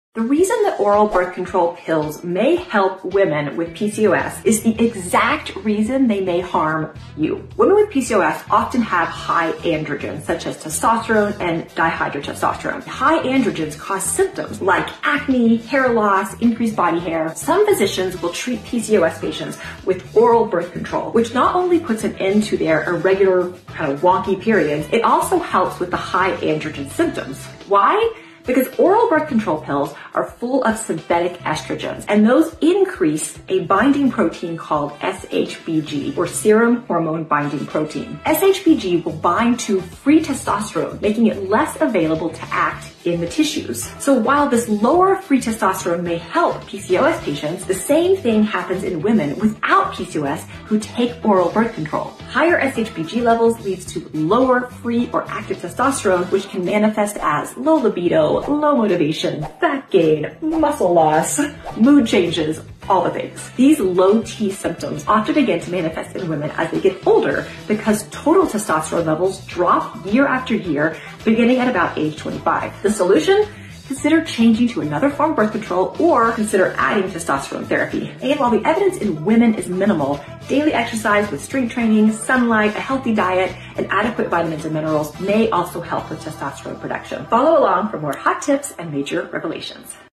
Oral estrogens can decrease your free testosterone by increasing SHBG
This is true for oral birth control and other oral estrogens.

I took birth control for years myself, having no idea that these hormones were affecting other hormones. 🤬

I prescribe oral estradial commonly, but understand that it may affect free T levels.

Whether this drop in testosterone causes symptoms varies from person to person as testosterone levels are actually pretty poorly correlated to symptoms in women. More on that soon! 🤯

Have you had low T symptoms with oral estrogens? ❓